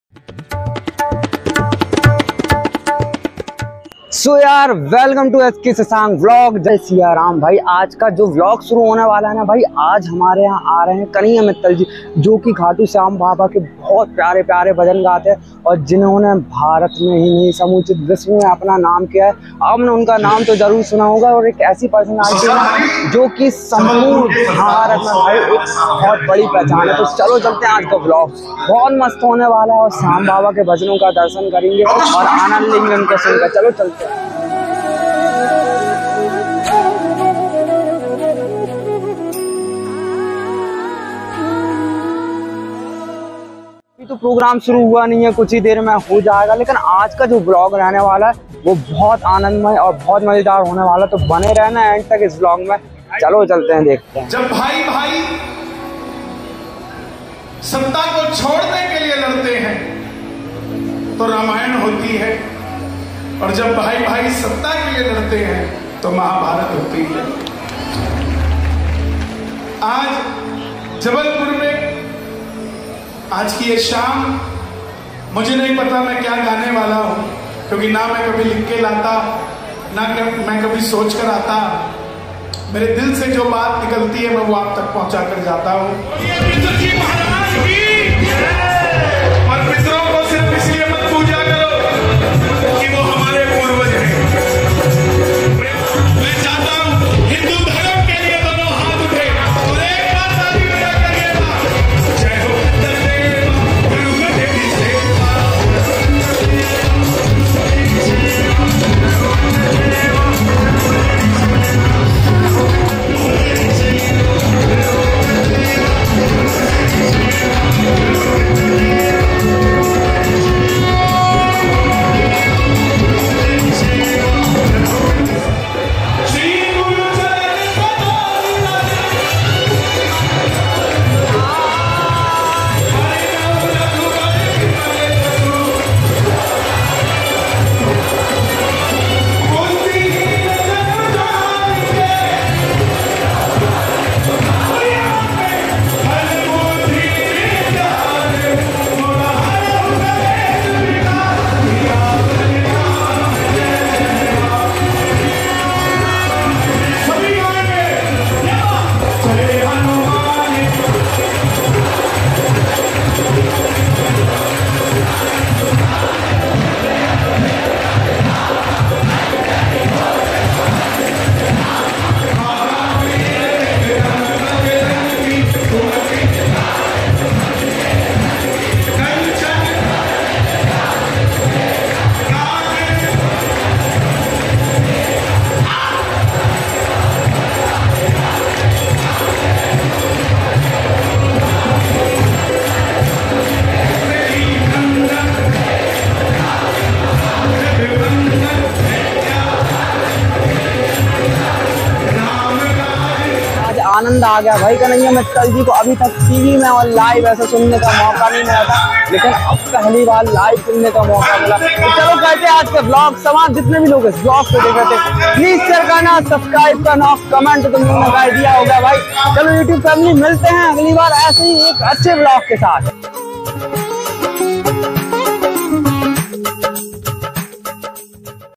संस्कार महोत्सव 🙏विश्व विख्यात भजन गायक कन्हैया मित्तल जी का आगमन❤️ खाटू श्याम भजन संध्या❤️
विश्व विख्यात मशहूर भजन गायक कन्हैया मित्तल जी का आगमन जबलपुर गोल बाजार पर श्याम बाबा के अद्भुत भजनों का रसखान साथिया कन्हैया मित्तल जी के कुछ ऐसे भजन सुनने का मौका मिला जरा देख कर सारा भारत मनमोहित हो जाता है और मंत्र मंगल हो जाता है साथी कन्हैया मित्तल जी के लाइव देखने का मौका मिला